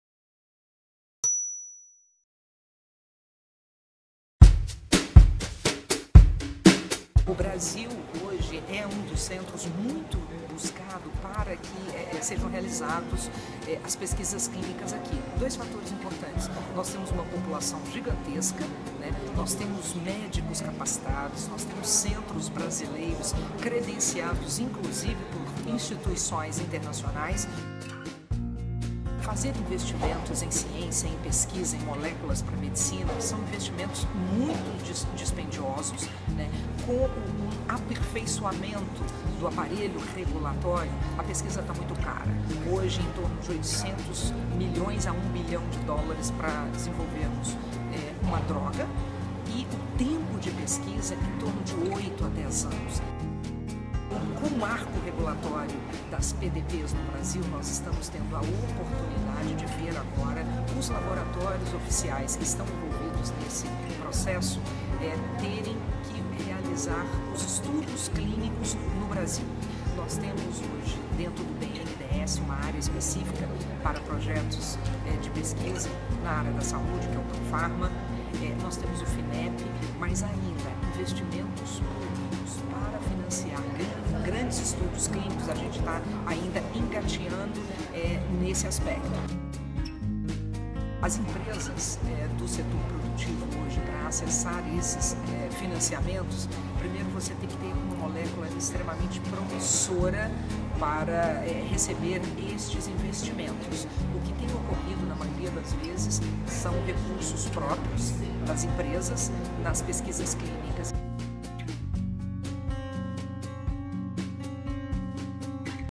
Gerente Geral da Genzyme vê Brasil atrasado em pesquisas farmacêuticas
Eliana Tamirão participou do Seminário Competitividade Setorial Saúde na sede da Amcham Brasil, em São Paulo.